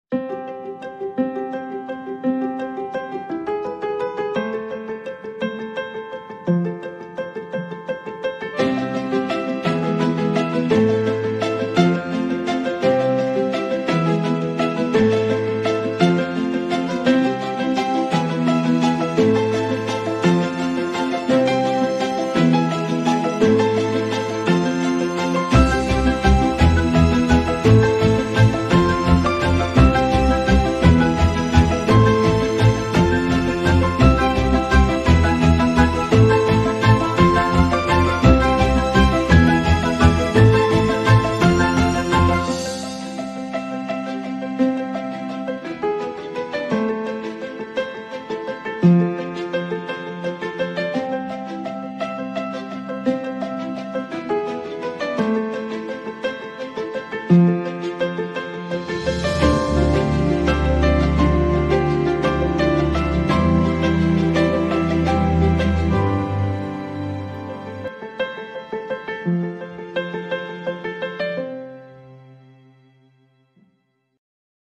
Pudu BellaBot - neue Generation Serviceroboter mit KI Sprachensteuerung und Katzengesicht
Eigenschaften der neuen Pudu Roboter: KI Sprachsteuerung und Gewichtssensoren.